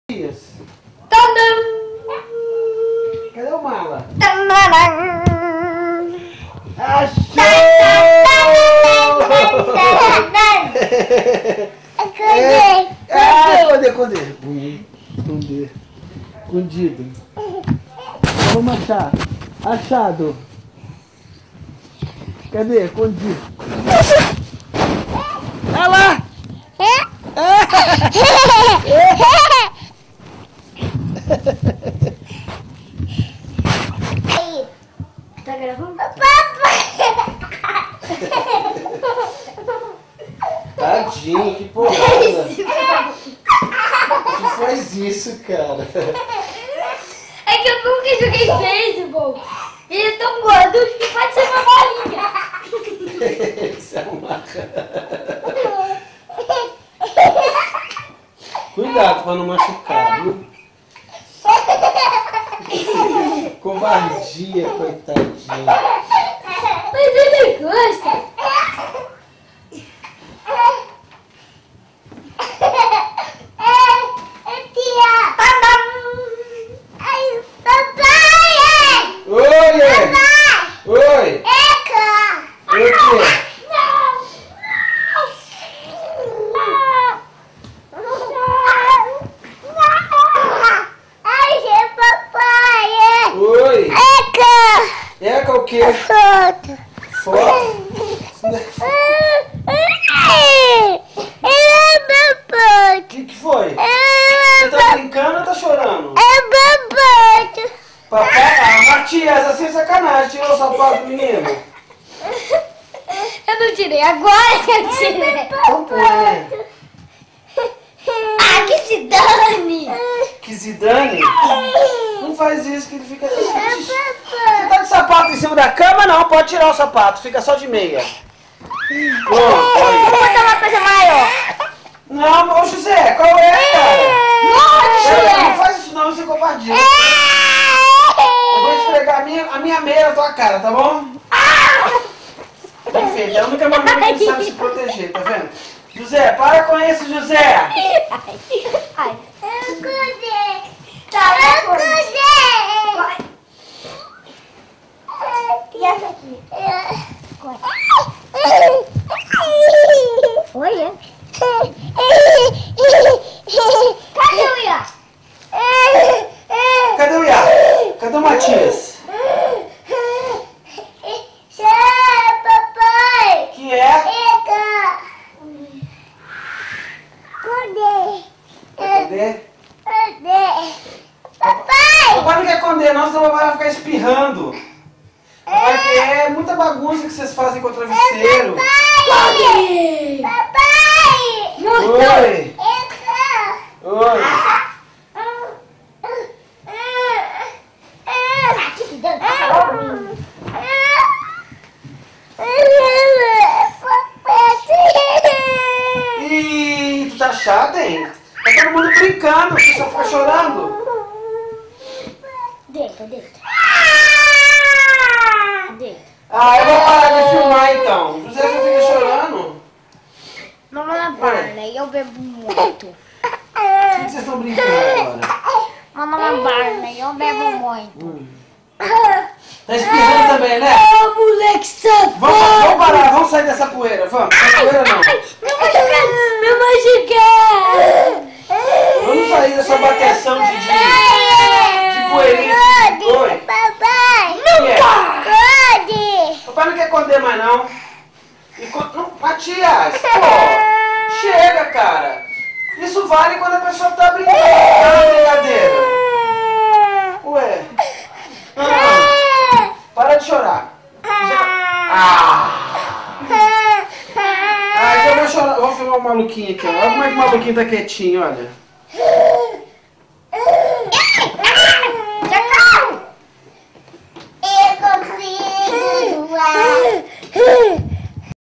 Matias e Jose em guerra de travesseiros
Matias Jose guerra travesseiros choros risadas provocações brincadeiras entre irmãos